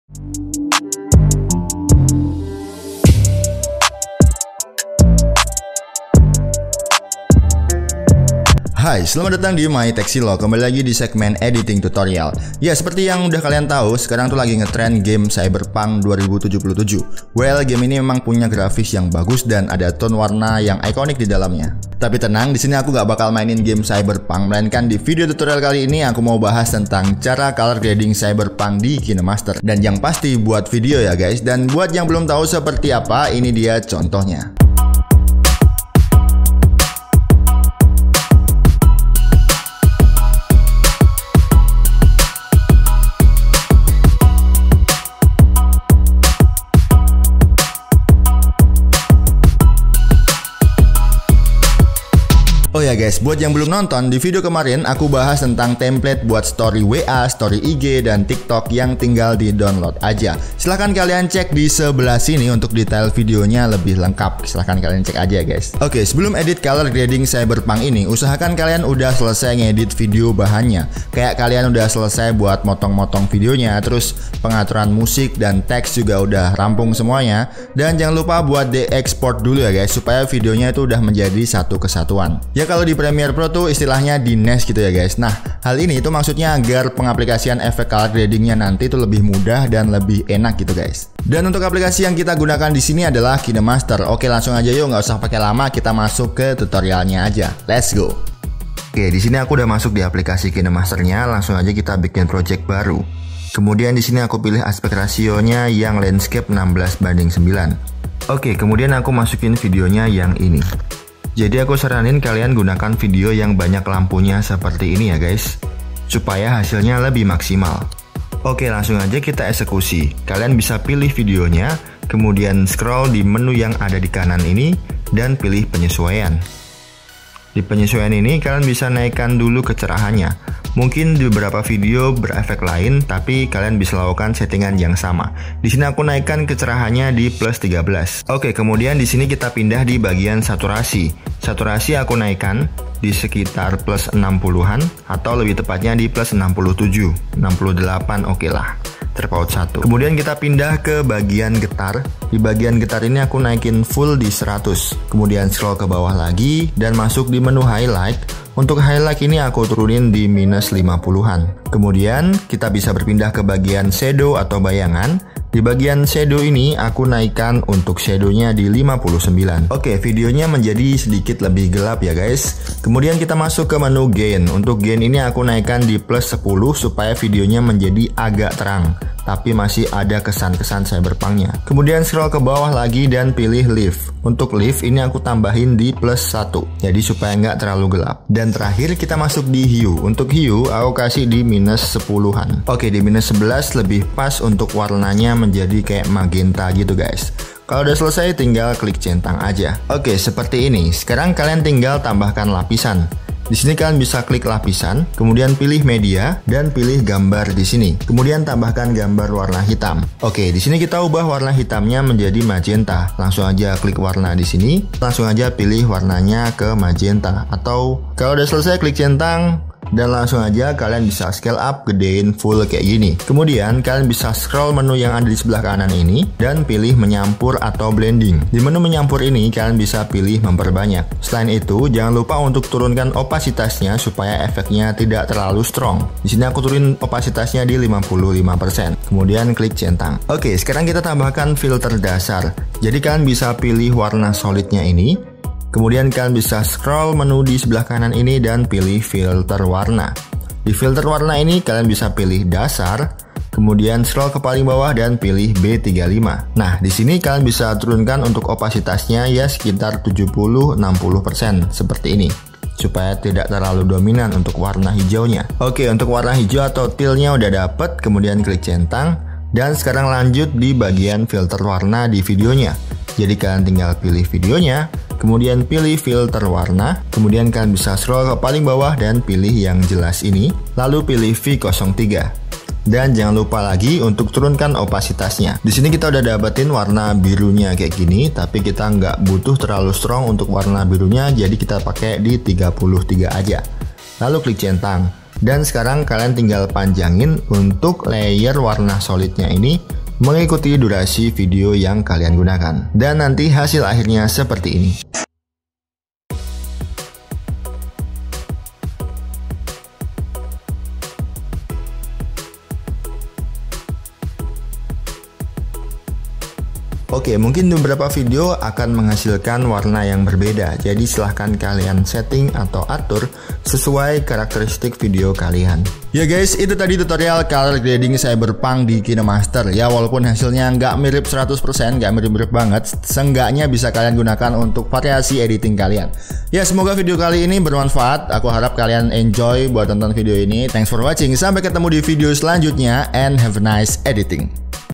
SIMPLE BANGET ! Grading Video Cyberpunk di Kinemaster android
Aku harap video ini berguna , bermanfaat untuk kalian semua.

Enjoy tutorial.